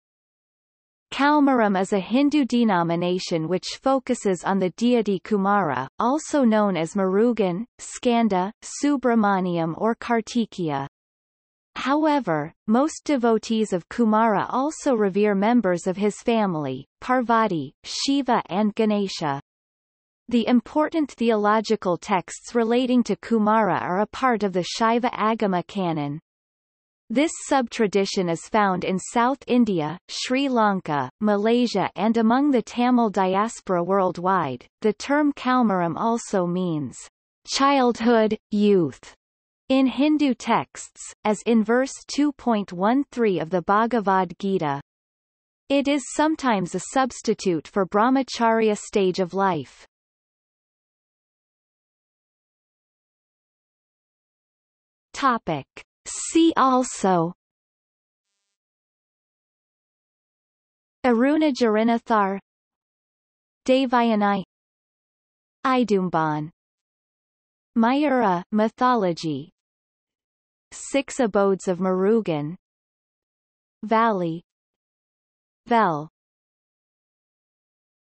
Kaumaram | Wikipedia audio article
00:00:51 See also

- Socrates

SUMMARY
Kaumaram is a Hindu denomination which focuses on the deity Kumara, also known as Murugan, Skanda, Subramaniyam or Kartikeya. However, most devotees of Kumara also revere members of his family: Parvati, Shiva and Ganesha. The important theological texts relating to Kumara are a part of the Shaiva agama canon. This sub-tradition is found in South India, Sri Lanka, Malaysia and among the Tamil diaspora worldwide.The term Kaumaram also means "childhood, youth" in Hindu texts, as in verse 2.13 of the Bhagavad Gita. It is sometimes a substitute for Brahmacharya stage of life.